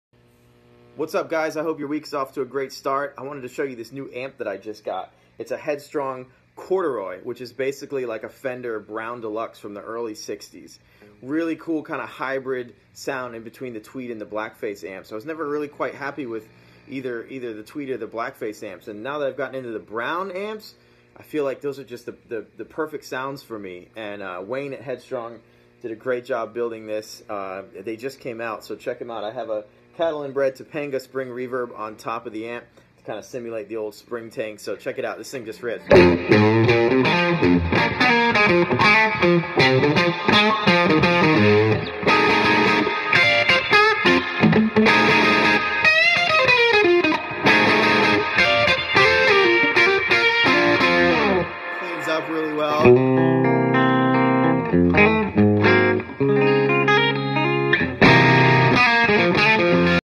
Headstrong Corduroy AKA 6G3 Brown Deluxe demo with Topanga Reverb pedal
DC native Matt Cummings based out of Nashville TN takes a minute (literally) to show off his new Headstrong, the Corduroy amp.  The Corduroy is a replica of the 1961-63 Fender 6G3 Brown Deluxe amp. 20 watts, 1X12 Eminence GA-SC64 speaker, two channels with Bias Vary tremolo just like the original.